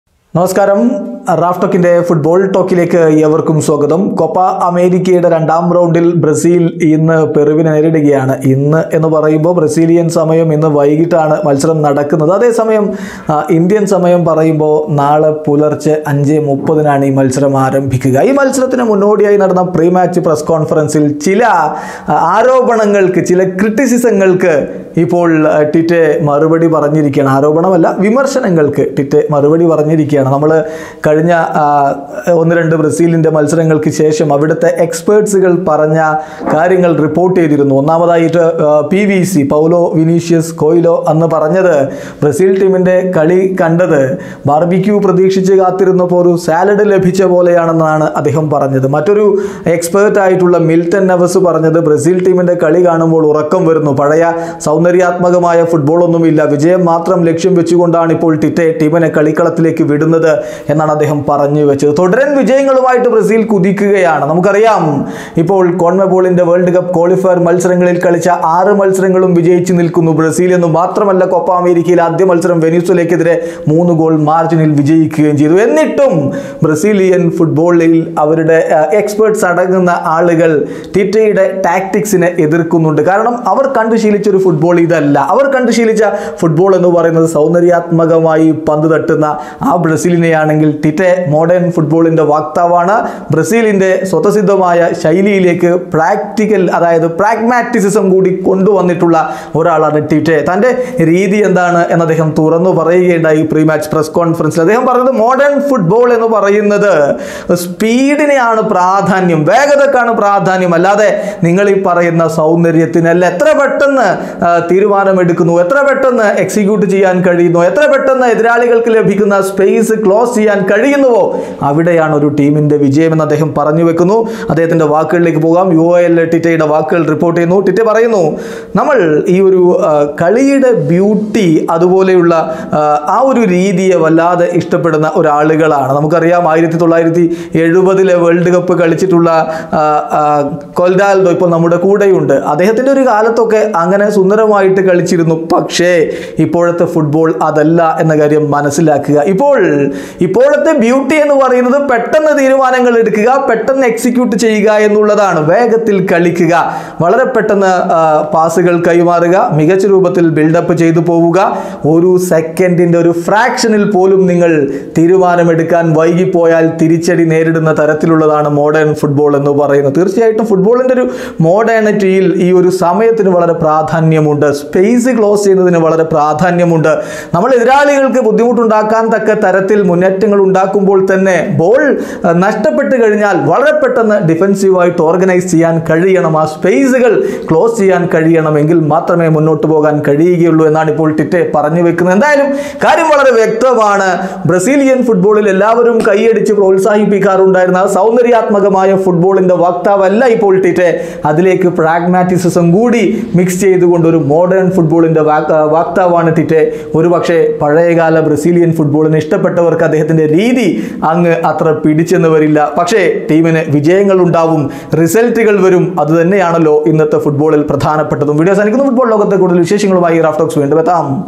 മോഡേൺ ഫുട്ബോളിൽ എന്ത് സൗന്ദര്യം? : വിമർശകർക്ക് ടിറ്റെയുടെ മറുപടി | Brazil vs Peru
Brazil vs Peru
Copa America